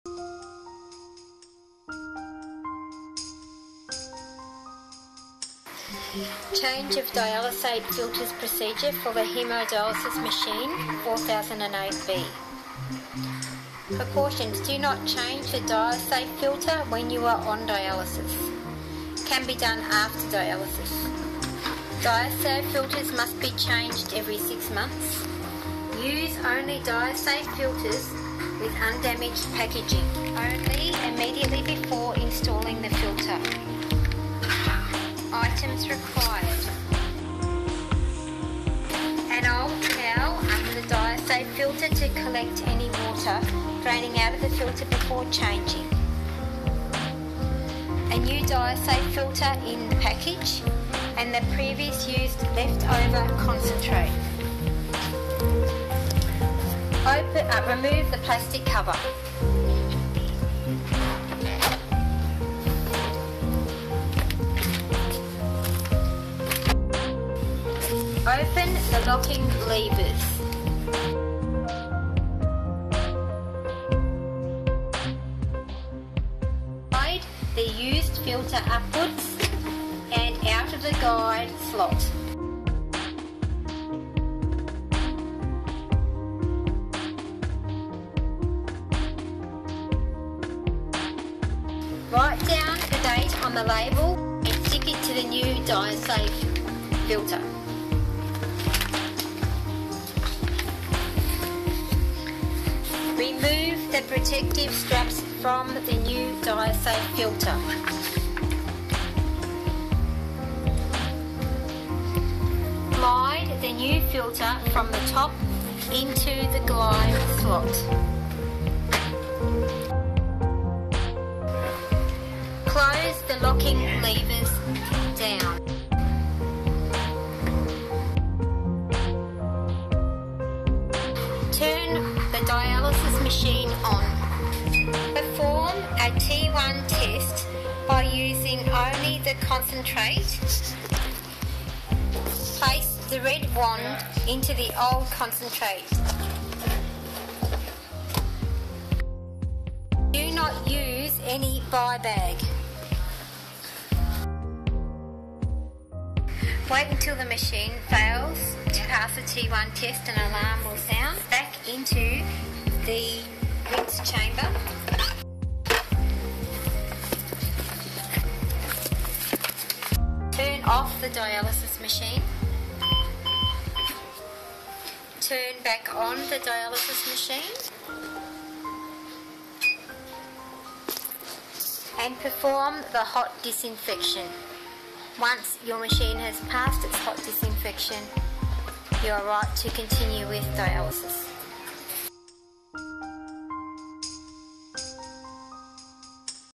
Change of Diasafe Filter on Fresenius 4008B.2.mpeg
Diasafe filter is changed manually and every 6 months for home haemodialysis clients using Fresenius haemodialysis machine 4008B.